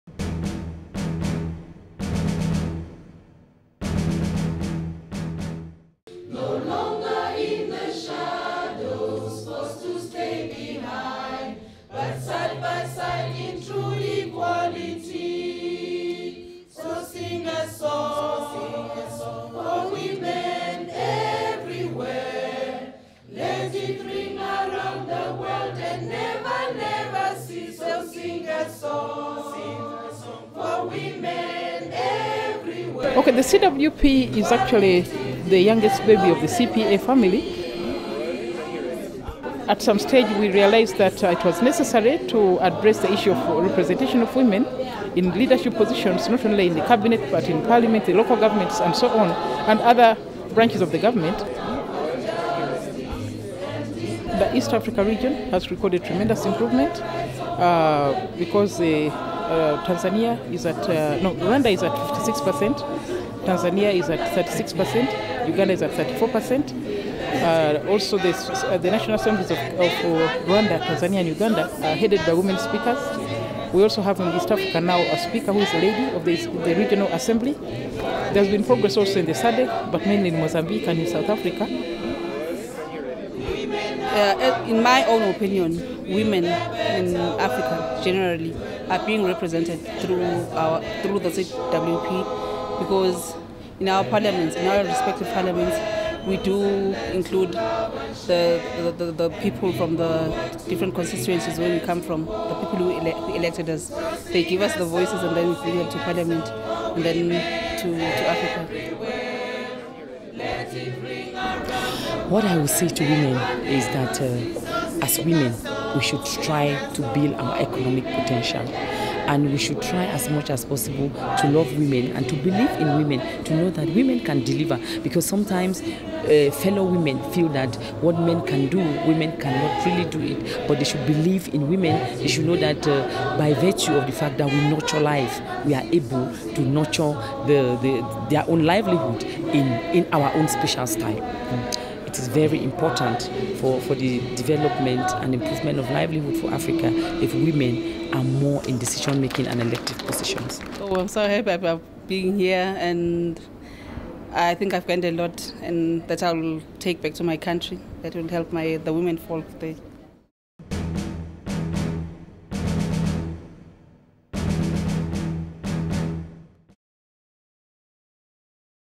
04 Day 4 Interviews with representatives for CWP at the 43rd CPA Africa Region Conference
Interviews with different representatives for CWP during the Sessions where they discussed programmes and progress of the CWP.